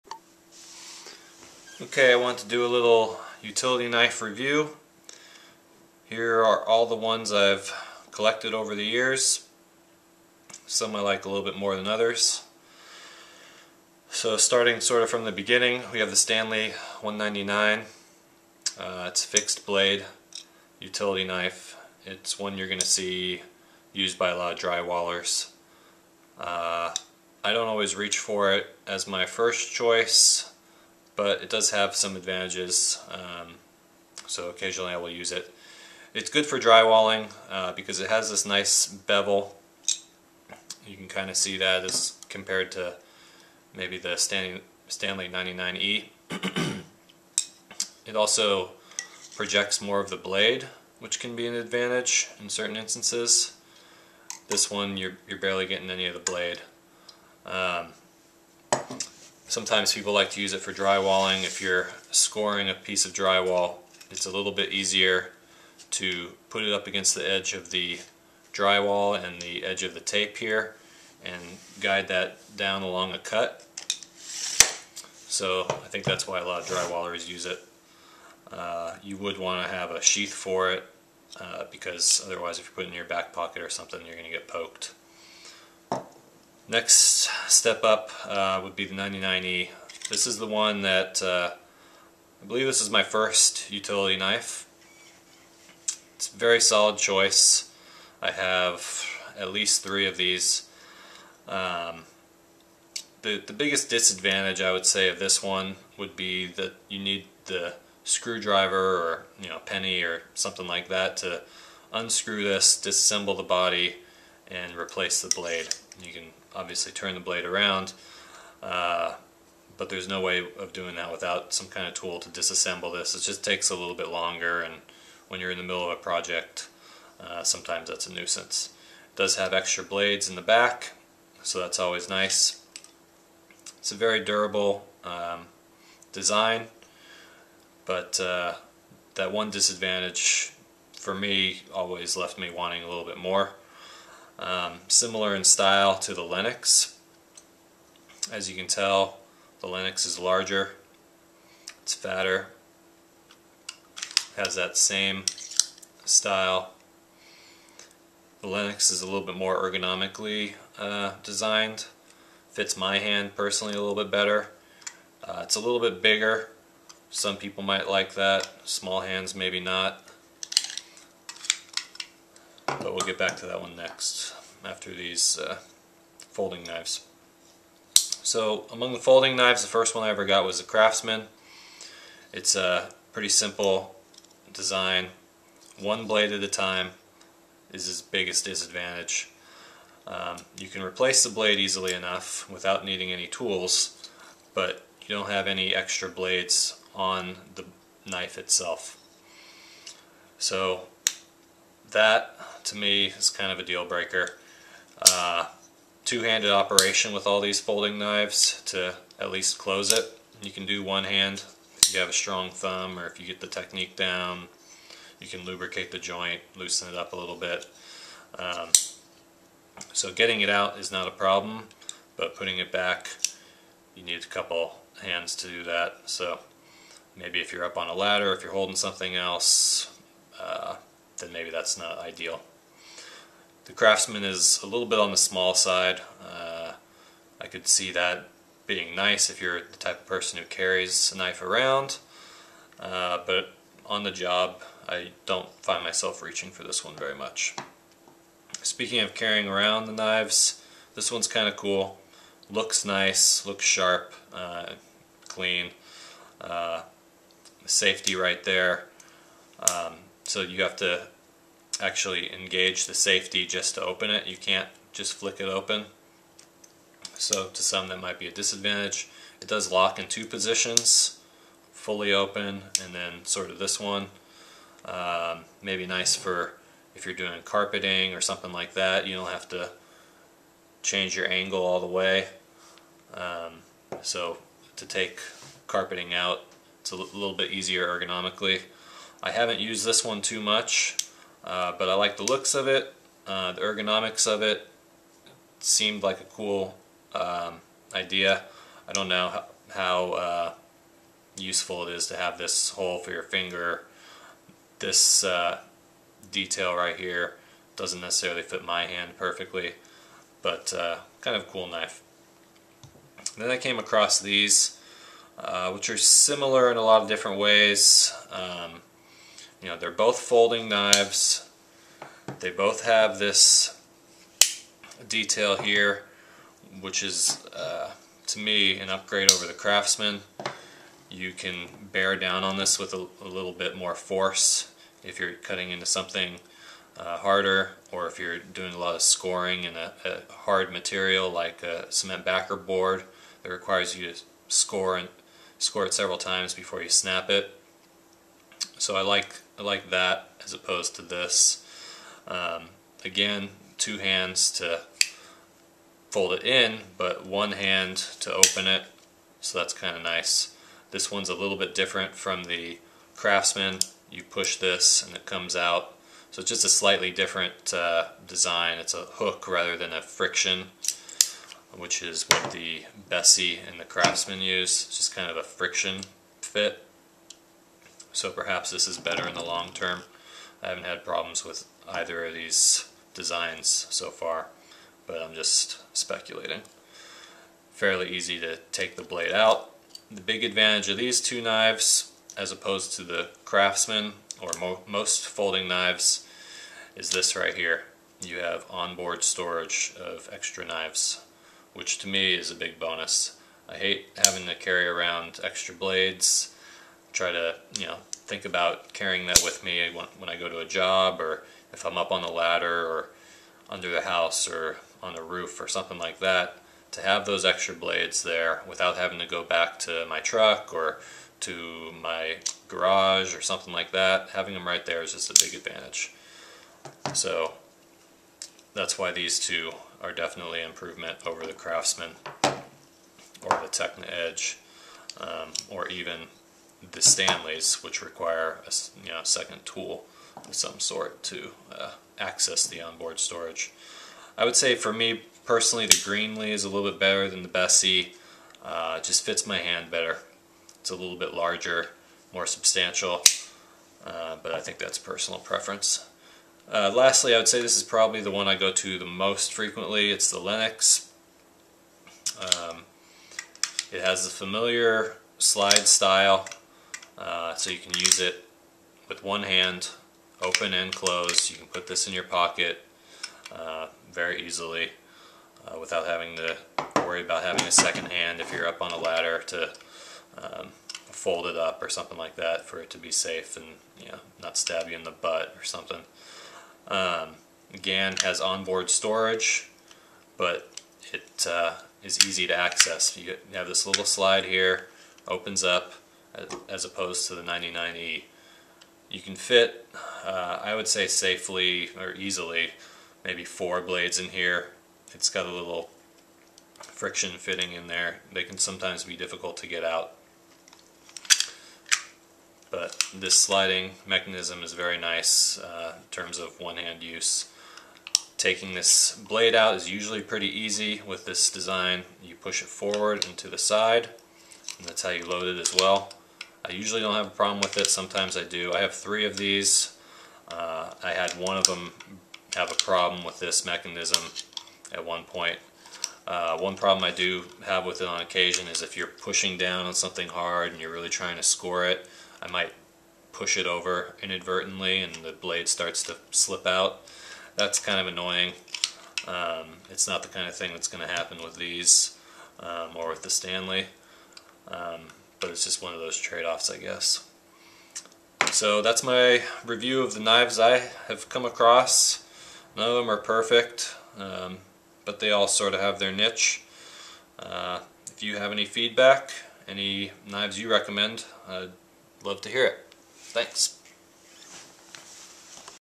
Utility Knife Review
My review of several common utility knives and styles.
Stanley 199
Stanley 99e
Craftsman folding knife
Bessey DBKPH
Lenox Gold
Greenlee 0652-23
Techni Edge REVO folding knife